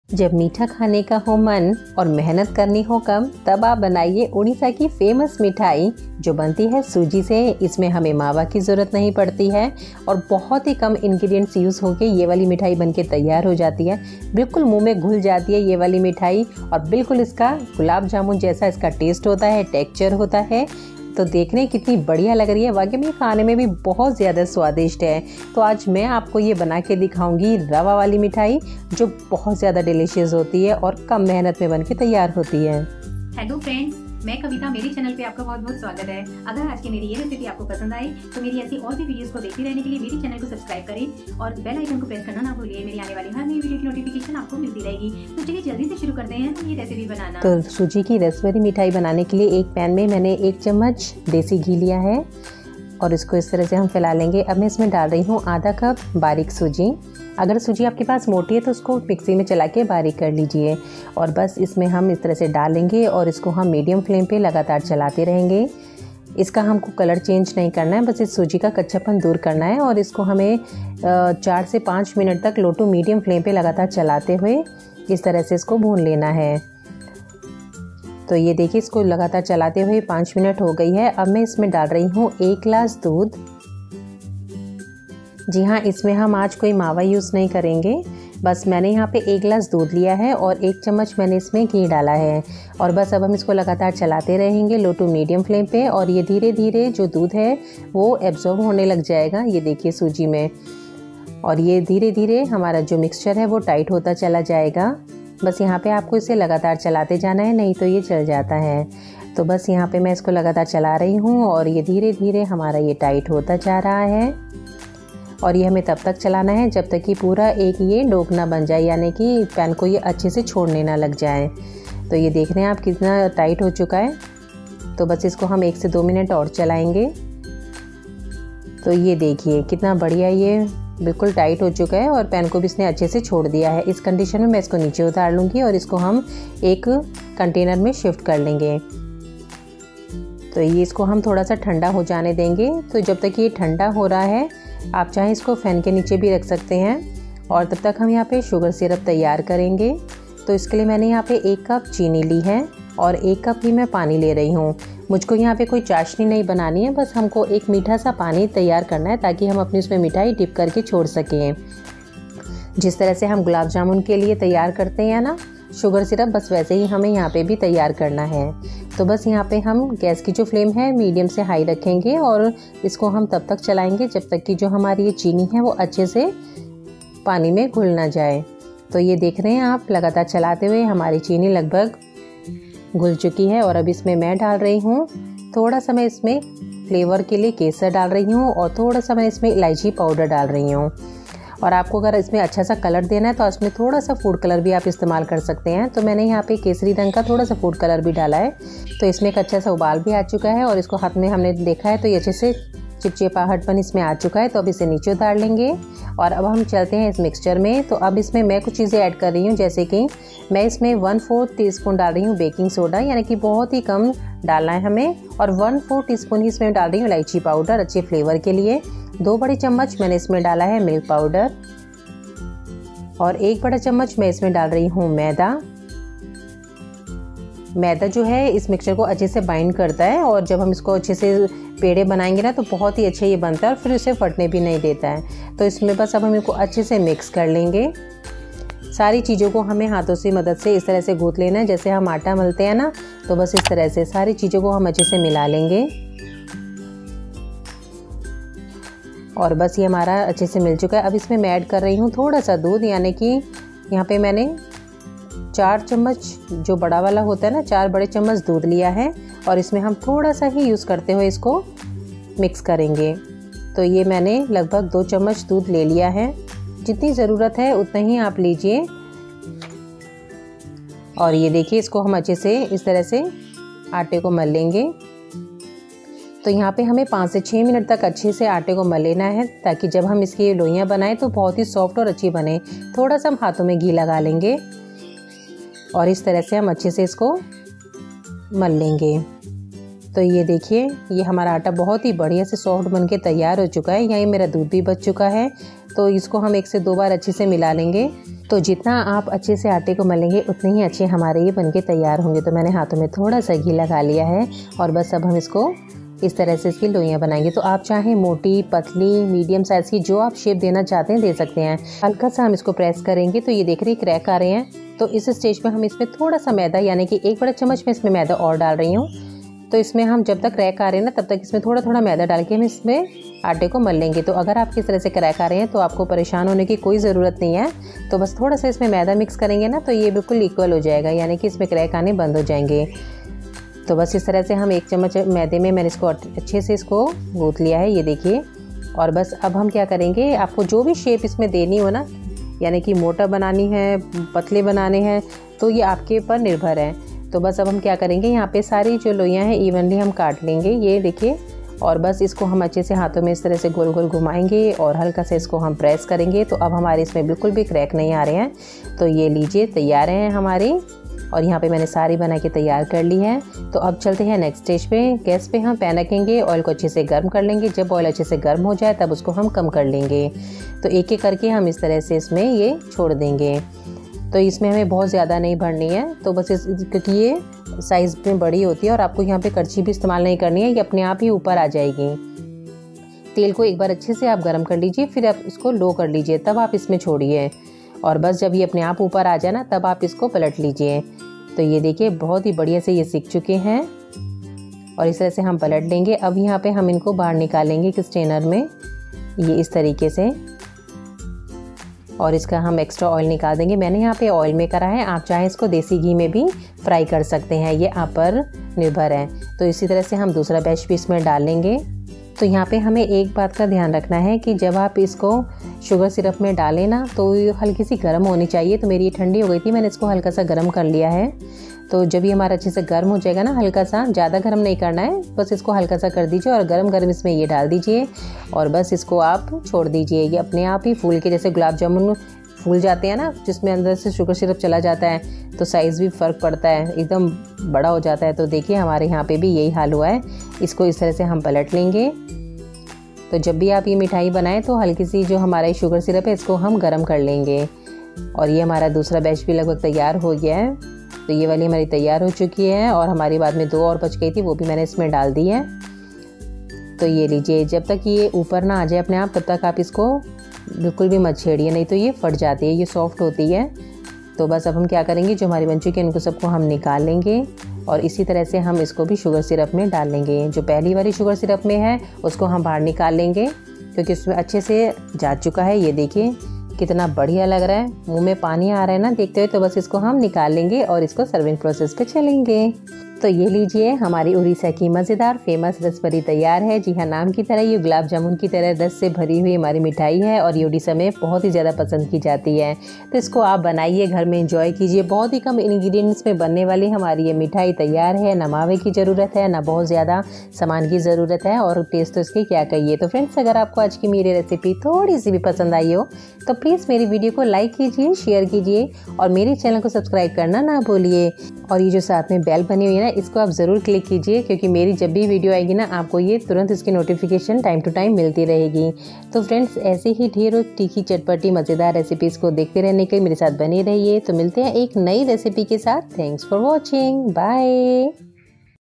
Rava rasbhari recipe | जब मीठा खाने का हो मन मेहनत करनी हो कम तब बनाए उड़ीसा की मिठाई रसभरी
Ingredients -
Saffron
Cardamom powder - 1/4 tsp
Food colour
Purpose flour - 2 tbsp
Oil
Follow me on your favourite social media channels:-
Important playlists that you should watch:-